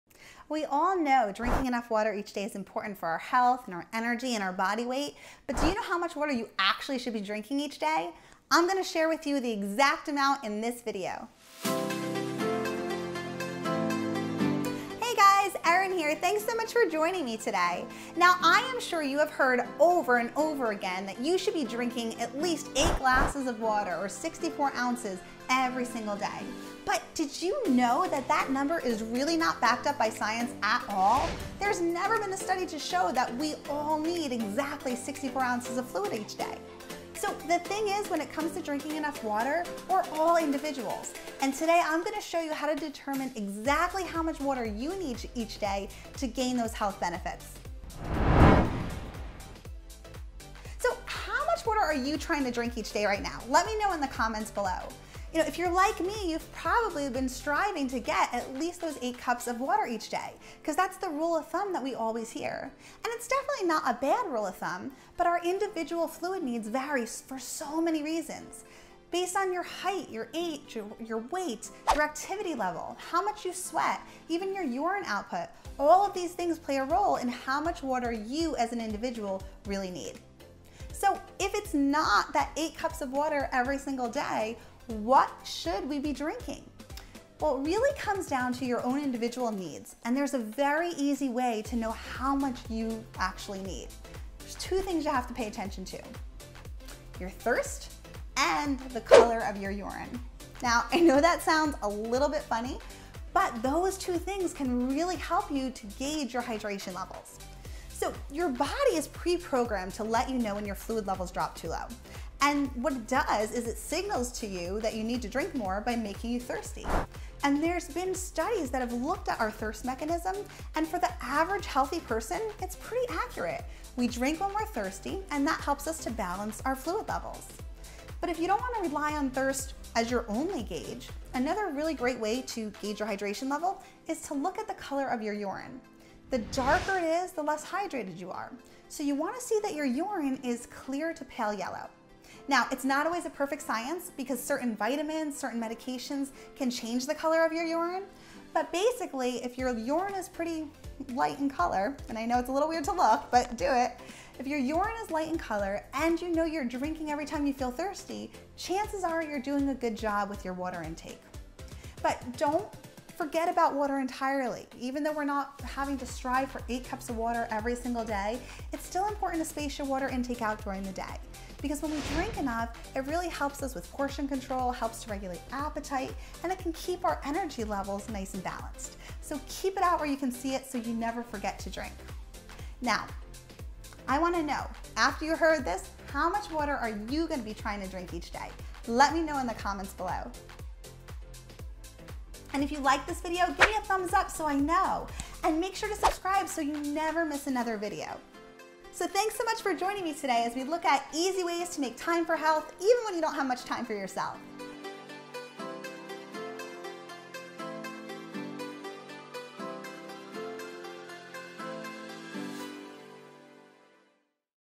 How Much Water Should You Really Drink Each Day
How Much Water Should You Really Drink Each Day. Drinking enough water is important for health, energy, and even weight management, but how much do water do you really need to drink each day? Registered dietitian and certified personal trainer Erin Palinski-Wade, RD, CDE answers that question in her latest ‘Healthy Mom Minutes’ video

Did you know that the recommendation to drink 8 cups of water per day is not based on any scientific research?! So how much water do you need to drink each day? The answer may surprise you.

Want more easy and healthy recipe ideas? Check out my blog for more tips & tricks you will love

So how much water do you really need each day?